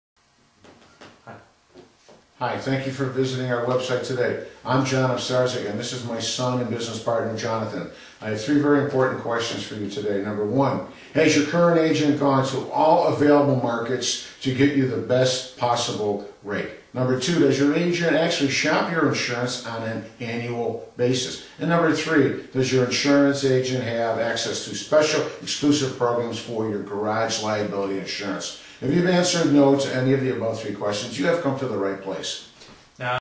Garage Liability Insurance
Insurance4MyGarage.com  - Video introducing Garage Liability Insurance for Insurance4MyGarage.com, a division of joinsurance.com